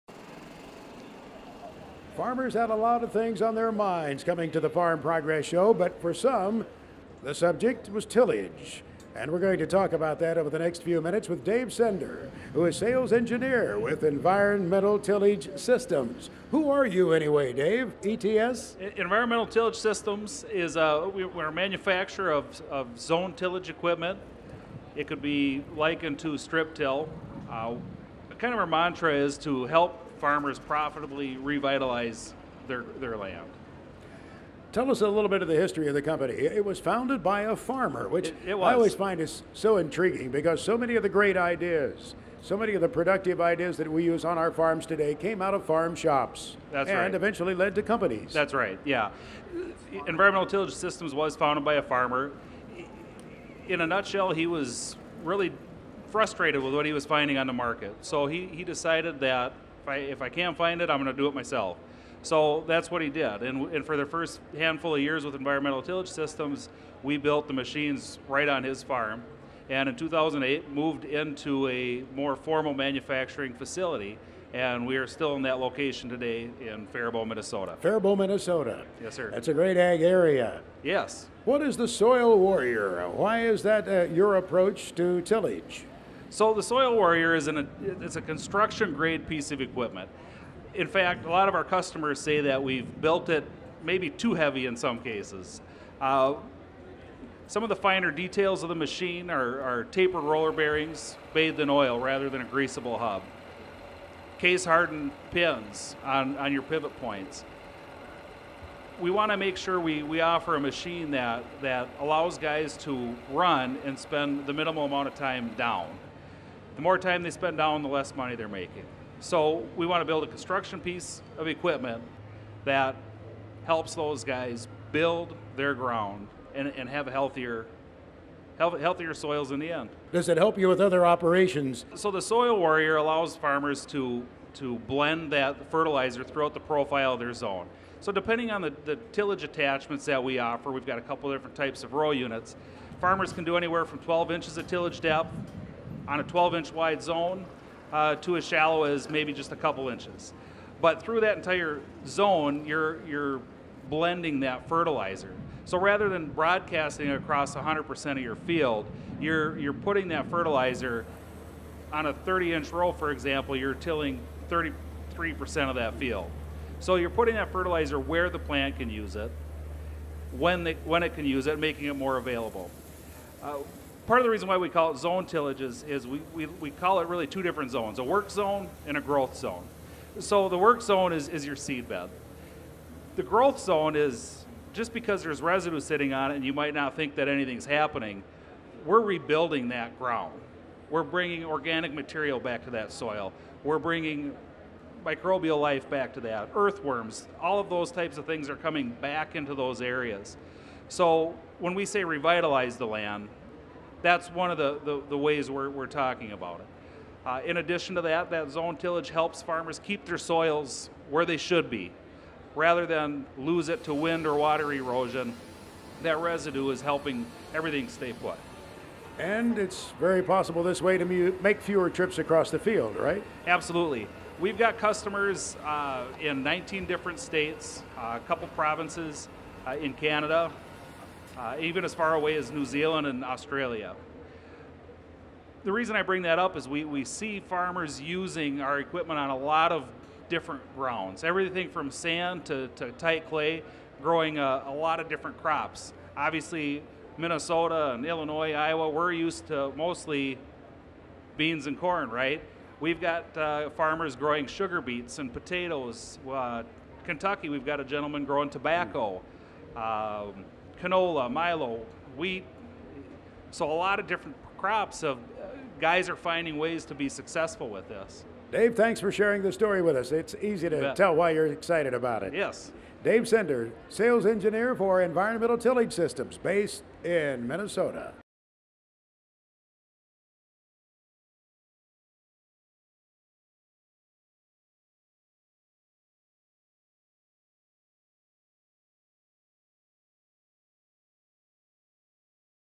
SoilWarrior Strip Tillage at Farm Progress Show 2015 | ETS SoilWarrior
Learn about ETS and the SoilWarrior strip-tillage system from the 2015 Best of Farm Progress Show. Discover how our innovative strip-till philosophy enhances precision agriculture, boosts soil health, and increases crop yields.